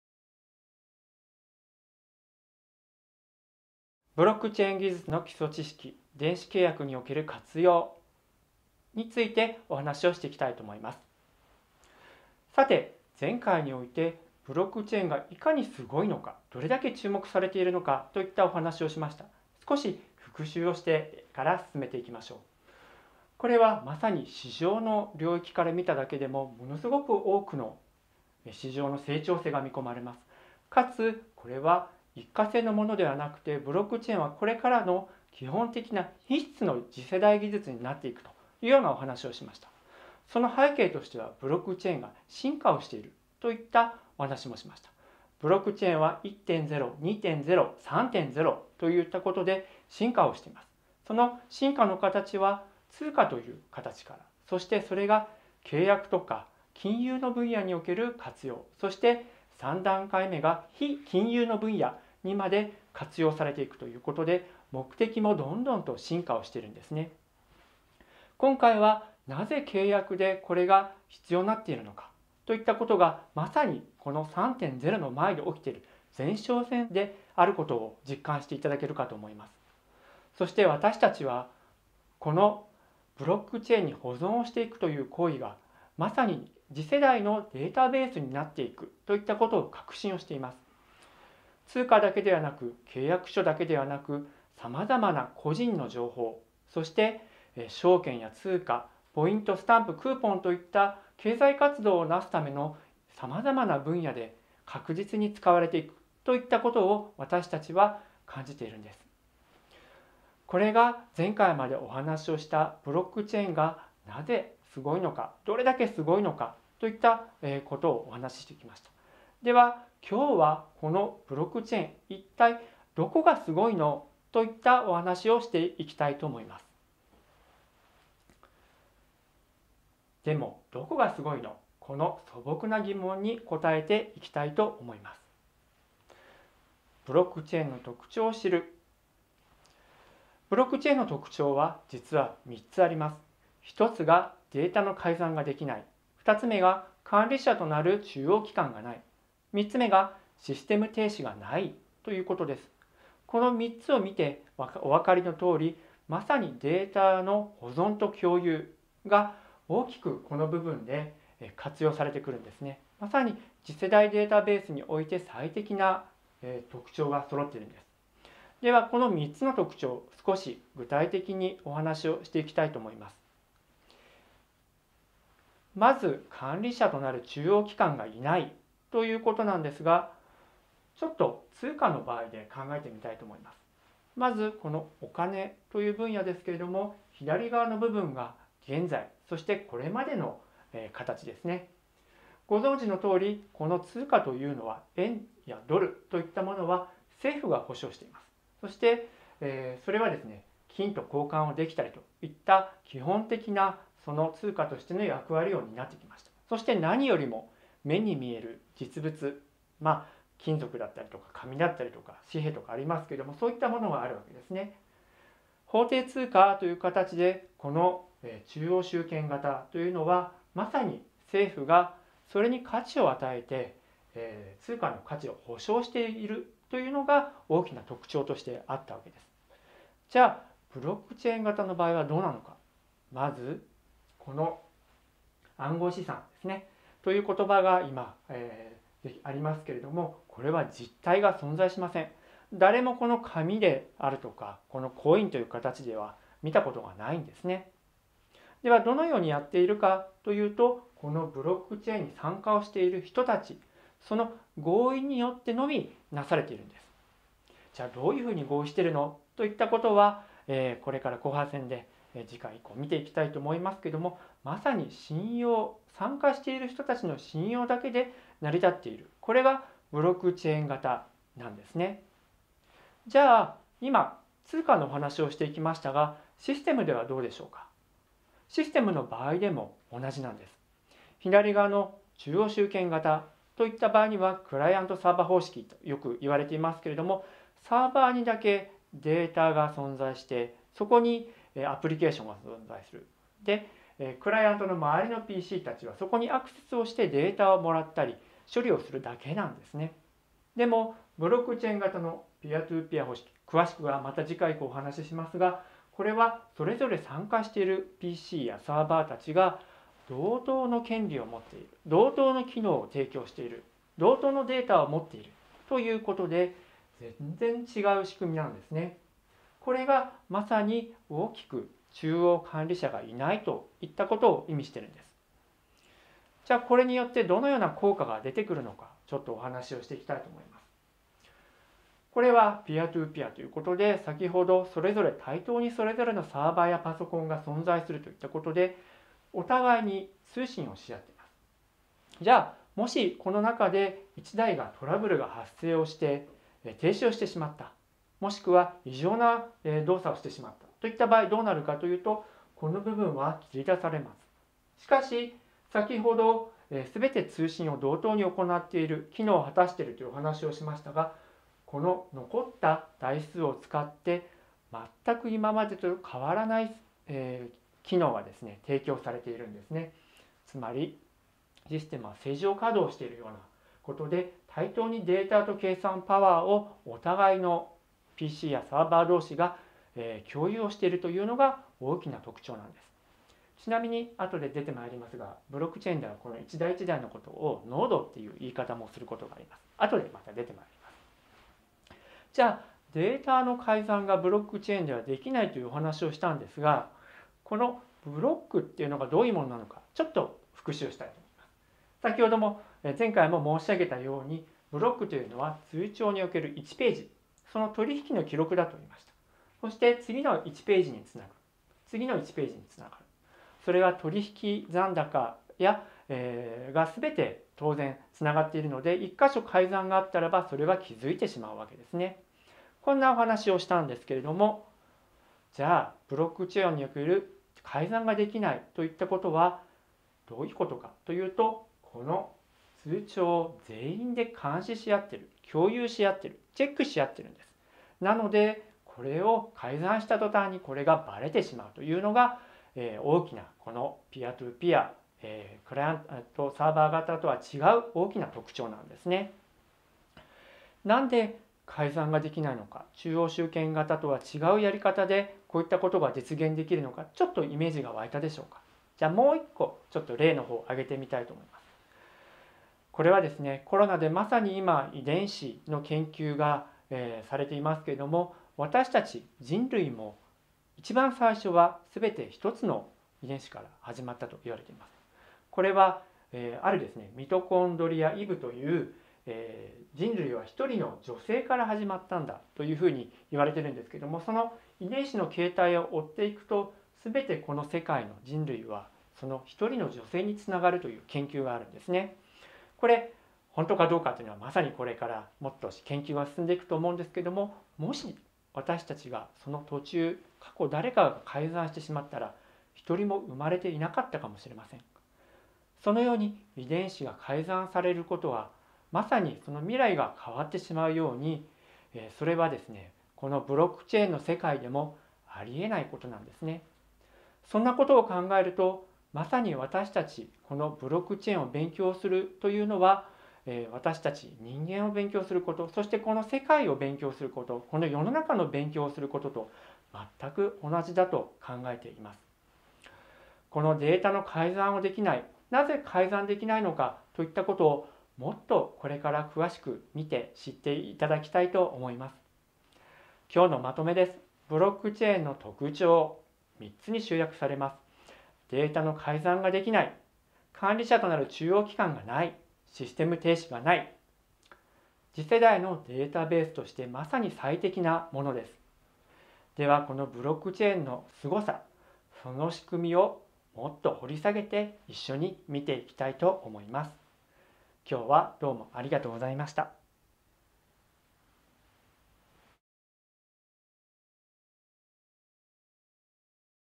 ブロックチェーンの基礎知識 3〜ブロックの特長〜
ブロックチェーン技術の基礎知識です。
AOSグループCTOの志田大輔氏の講演内容です。全体では、１０部で構成されています。
1 . イントロダクション 真贋判定システム
2. ブロックチェーンとは
3. ブロックチェーンの特長
4. ブロックチェーンの仕組み -分散台帳、合意形成(ハッシュ)
5. ブロックチェーンの仕組み -合意形成(マイニング、コンセンサスアルゴリズム)
6. ブロックチェーンの仕組み -暗号技術
7. ブロックチェーンの仕組み -スマートコントラクト
8. ブロックチェーンの活用事例
9. 電子契約におけるブロックチェーンの活用事例
10. エンディング